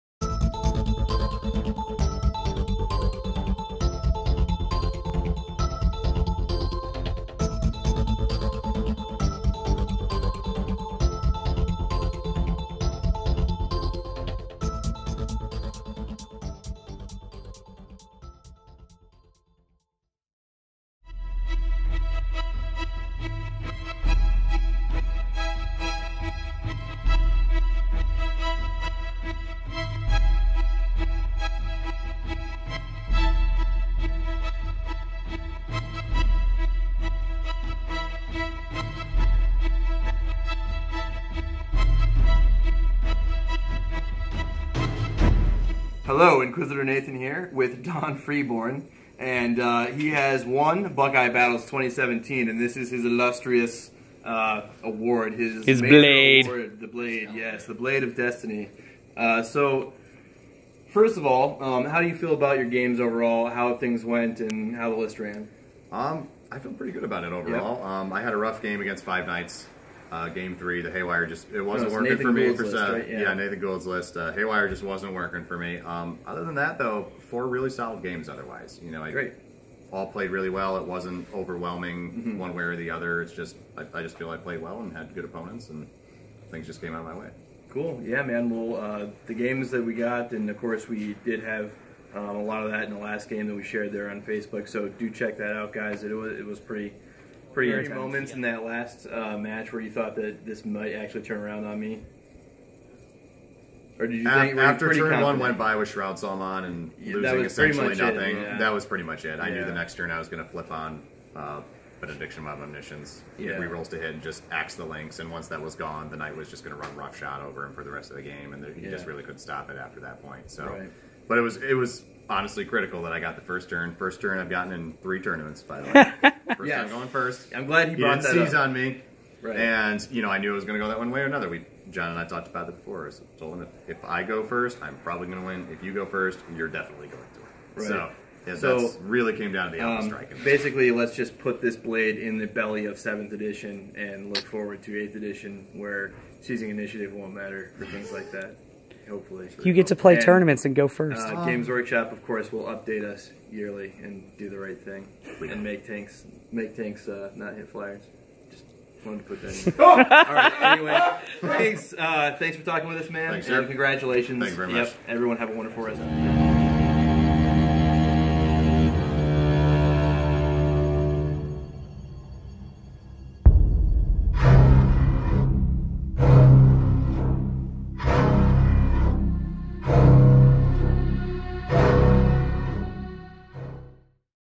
Buckeye Battles 2017 Winner Don Freeborn Interview
Winner of Buckeye Battles 2017 Exclusive Interview with player Don Freeborn